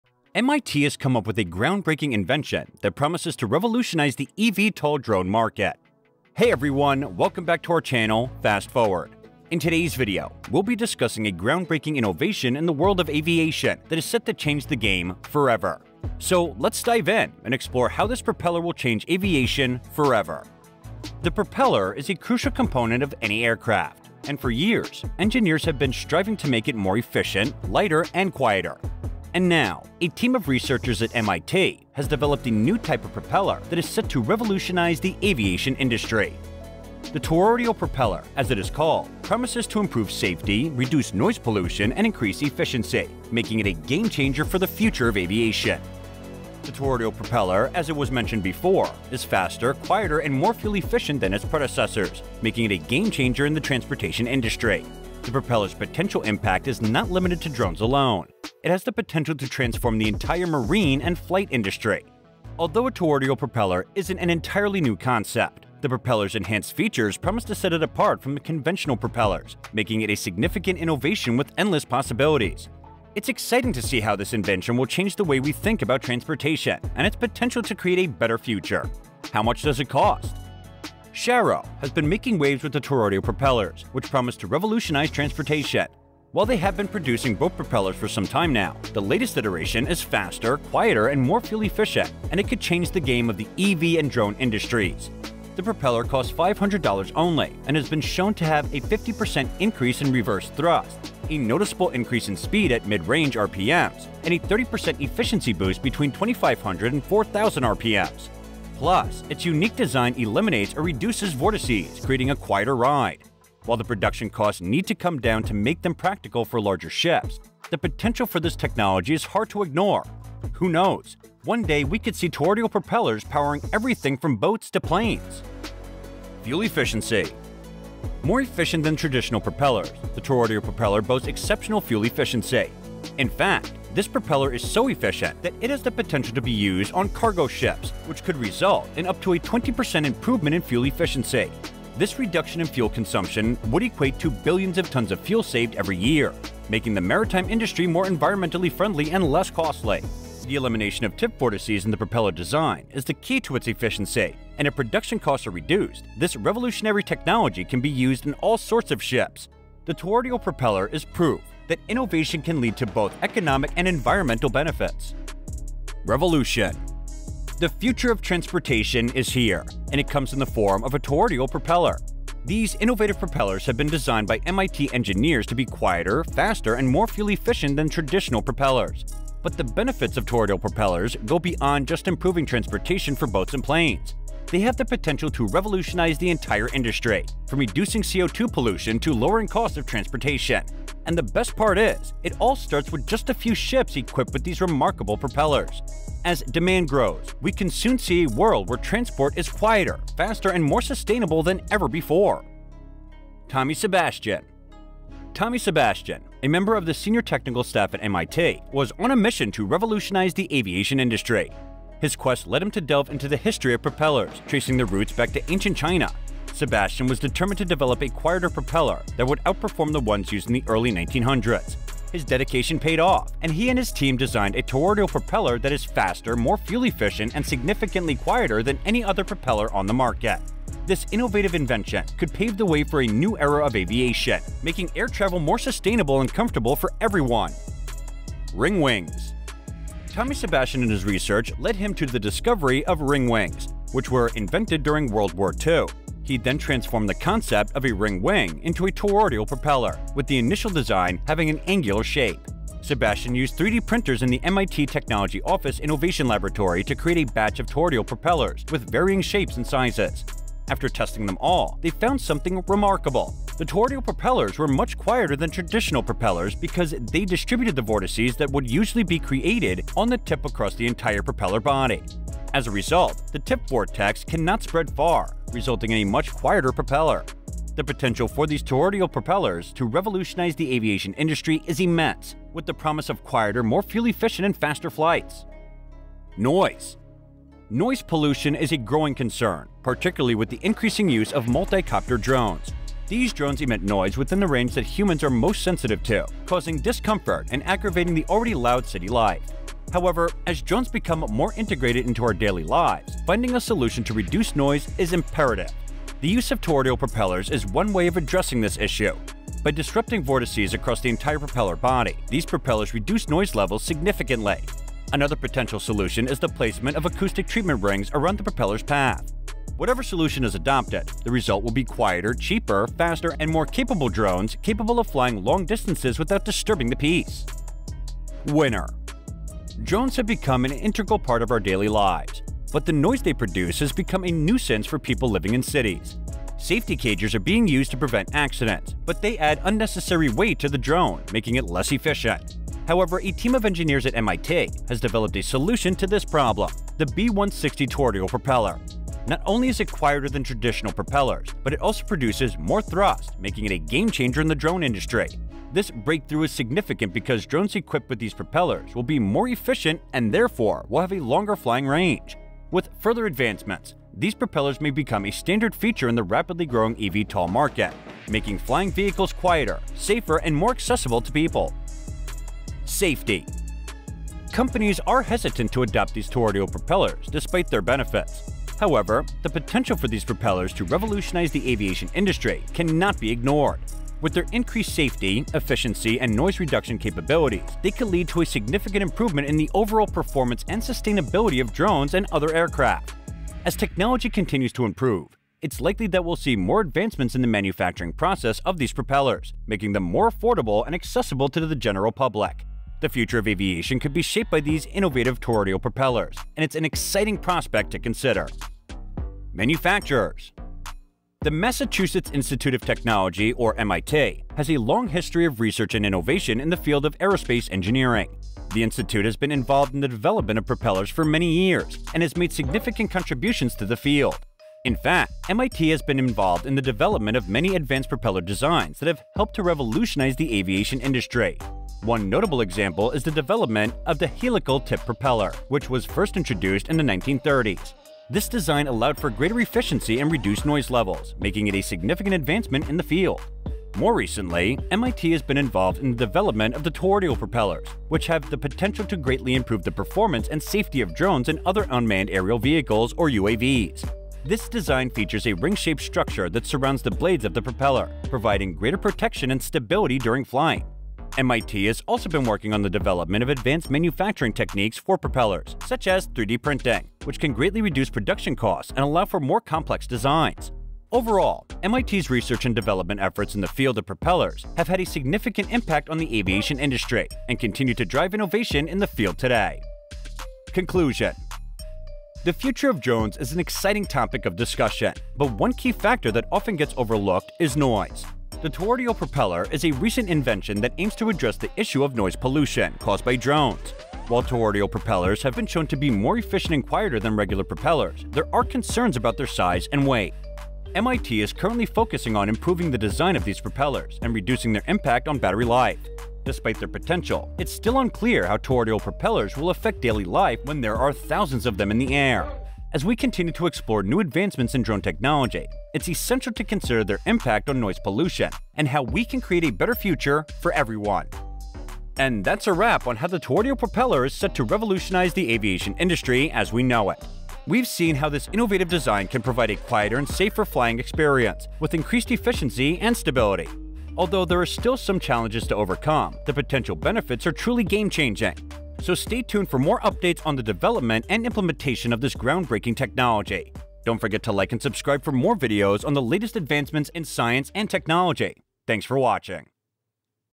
This Propeller will Change Aviation Forever!
Are you curious about how propellers work? Do you want to know what makes them so special? Then you need to watch this video!

In this video, we'll take a look at the revolutionary Toroidal propeller – a propeller that will change aviation forever! With this new propeller, planes will be able to travel much faster and quiter than ever before.

Fast Forward brings you the latest news in technology, science, cosmology, innovation and futurism. New videos are uploaded every week.